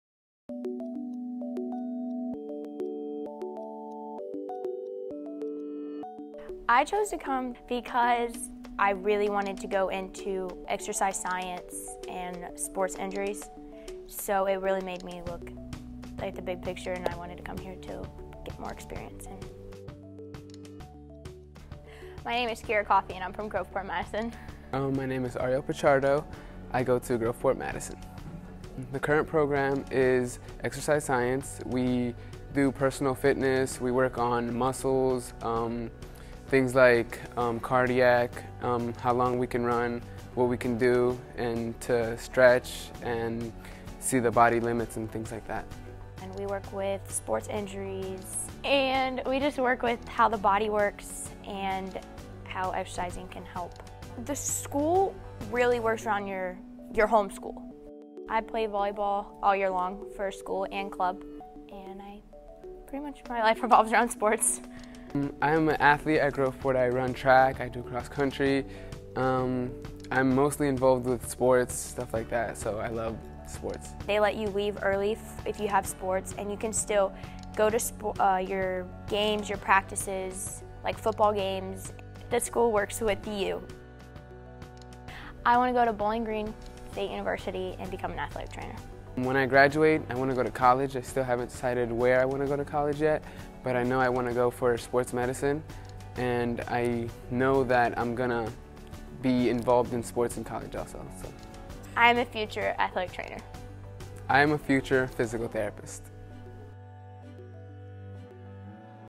Exercise Science
Jump into the in-demand field of exercise science. The Exercise Science program is for the student who is interested in helping people prevent injuries or working with patients to help relieve their pain and increase their strength and mobility.

Put your good foundation in science to work for you as you promote health and help patients. The Exercise Science program includes studies in:

    Health education
    Health and fitness
    Anatomy
    Physiology
    Exercise programming and prescription
    Safety
    First aid
    Rehabilitation and mobilization of injury
    Massage techniques and relaxation
    Nutrition and diet implementation
    Sports psychology
    Career exploration
    Preparation for personal training exam